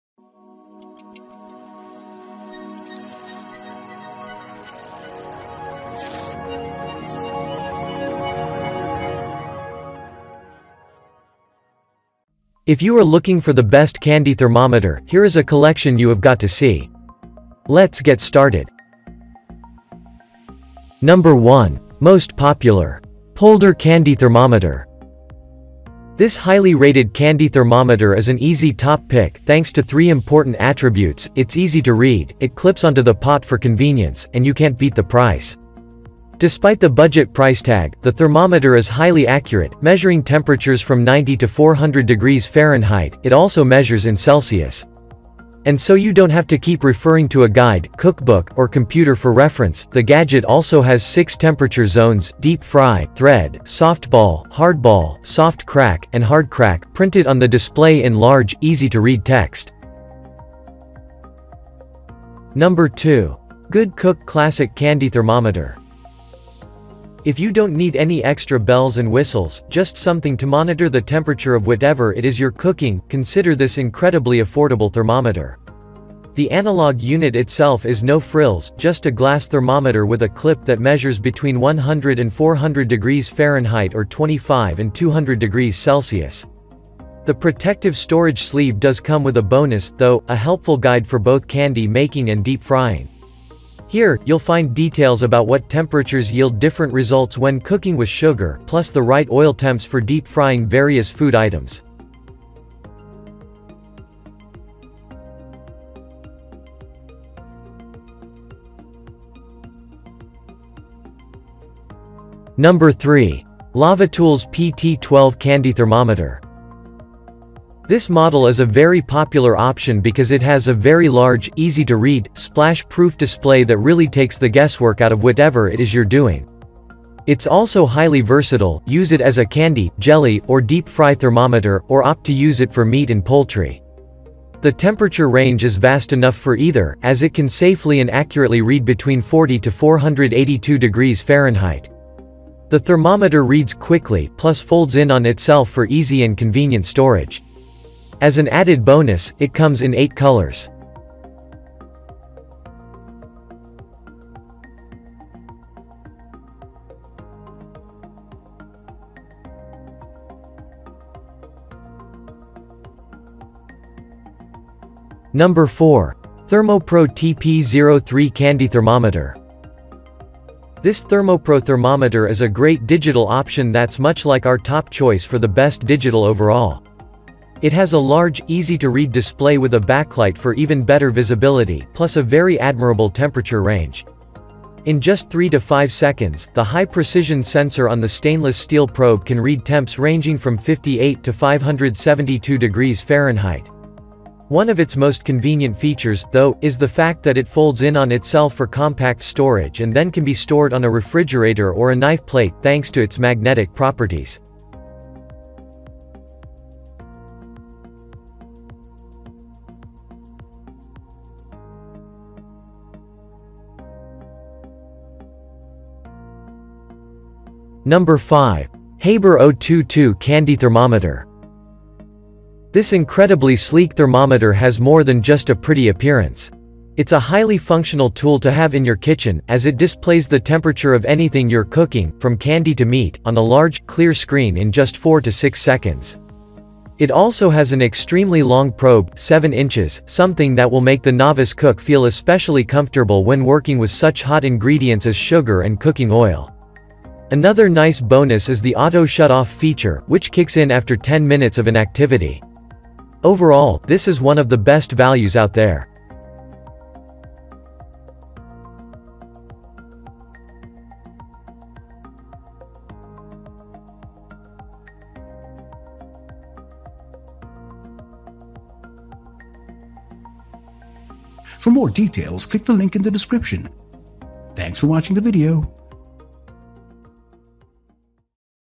Top 5 Best Candy Thermometers Review in 2023
Best Candy Thermometers featured in this Video:
0:21 1. Polder Candy Thermometer
1:02 2. GoodCook Classic Candy Thermometer
1:52 3. Lavatools PT12 Candy Thermometer
2:06 4. ThermoPro TP03 Candy Thermometer
3:38 5. Habor 022 Candy Thermometer

What Is A Candy Thermometer?
A candy thermometer is a cooking thermometer used to quantify the temperature and along these lines the phase of a cooking sugar arrangement. These thermometers can likewise be utilized to quantify hot oil for profound broiling.

What is the Best Candy Thermometer?
We strive to review as many products as we can, read real-time reviews, and made our own ranking. We welcome all your comments.